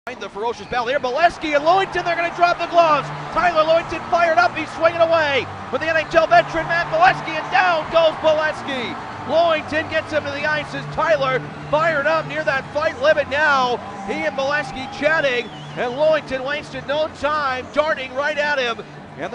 Matt Beleskey vs Tyler Lewington Mar 25, 2018
Matt Beleskey vs Tyler Lewington from the Hartford Wolf Pack at Hershey Bears game on Mar 25, 2018.